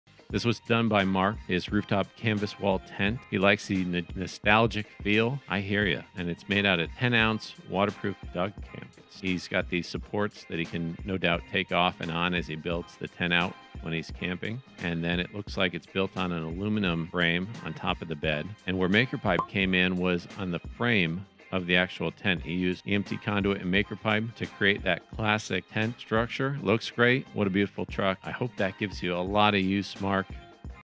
DIY Classic Tent Build
Mark from our community wanted to recapture that nostalgic camping in a tent for this summer for his family. It Looks amazing!